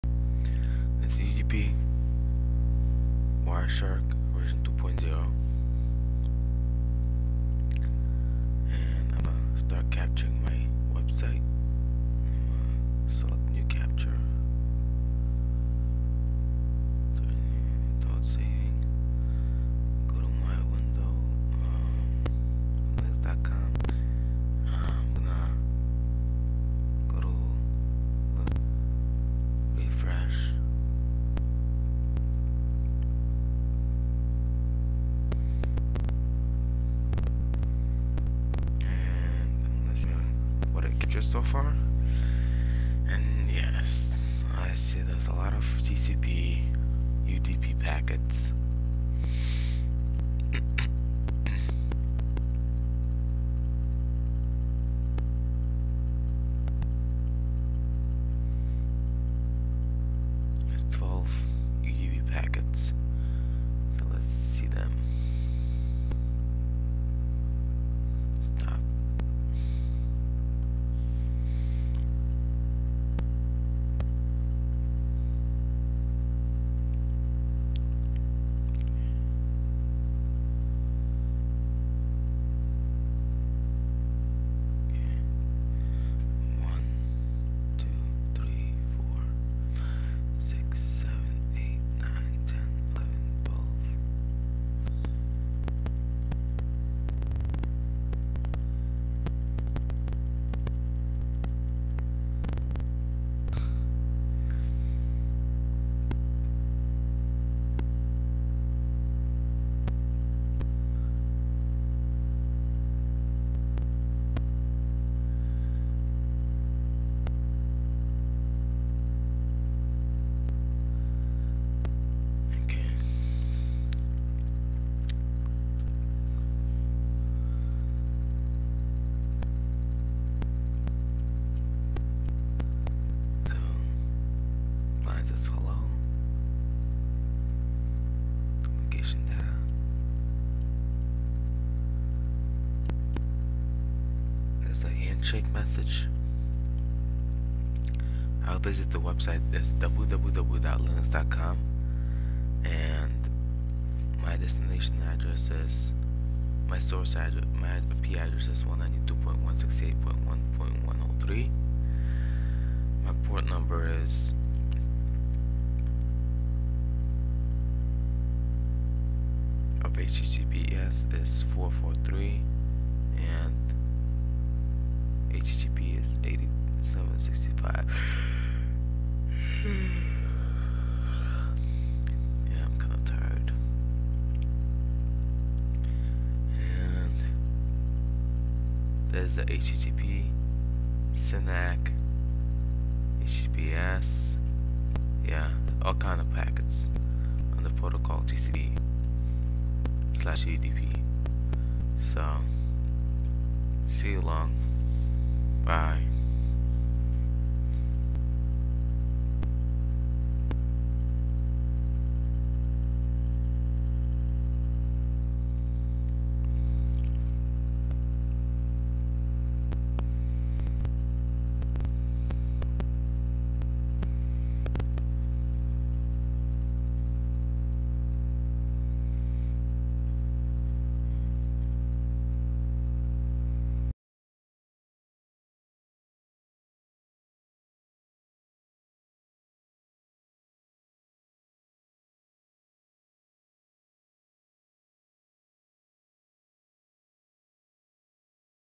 Wireshark UDP v2.0 by Ruslan Glybin.avi
The four fields in UDP header are Source Port,Destination Port,

Length, and Check-sum. The UDP header has four fields each source

port number 2 bytes, destination port number 2 bytes, datagram

size(Length) 2 bytes, and check-sum 2 bytes. Length field is is a

count of number of bytes contained in the header and data section.

The value in the Length field is 37. The maximum size of a datagram

depending on the operating environment with a two-byte size field, the

theoretical maximum size is 65535 bytes. The biggest source port

number is 65535. The number protocol UDP is 0×11hexadecimal and 17

decimal. The Pseudo Header Field is used in calculation check-sum.

First packet source port is 8730, Destination port is 443. Second packet

source port is 443, Destination port is 8730.